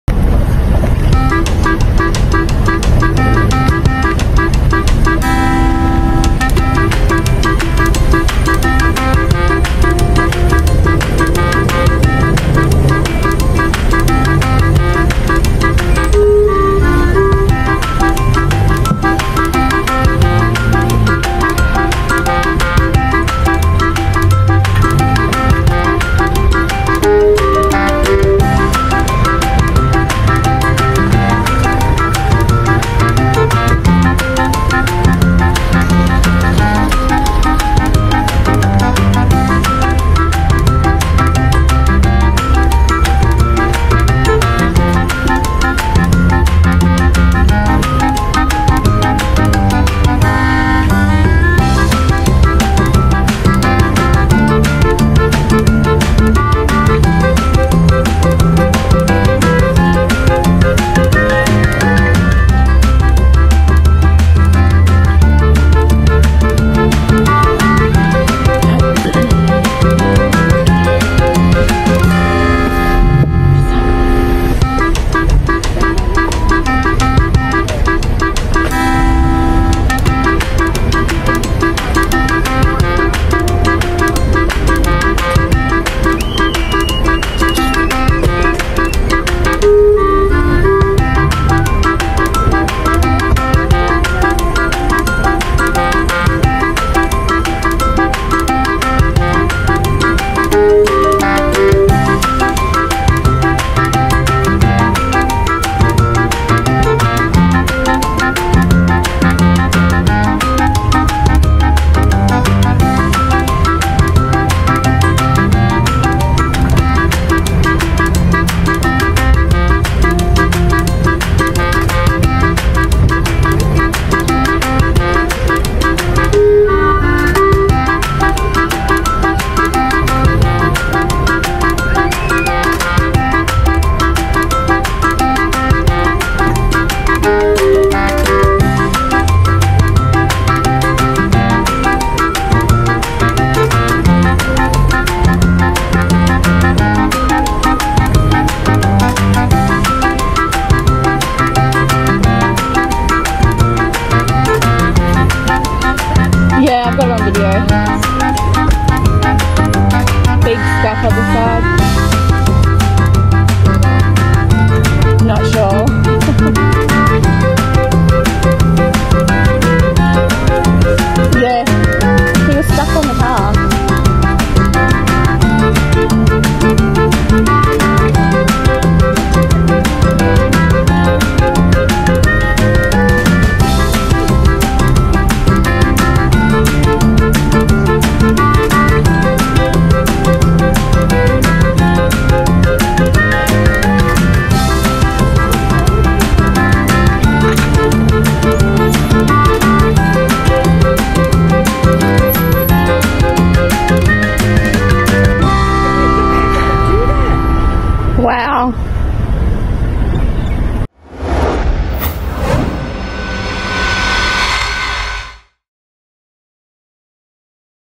Wyndham's Worst Drivers
Check out this parking effort in Point Cook recently!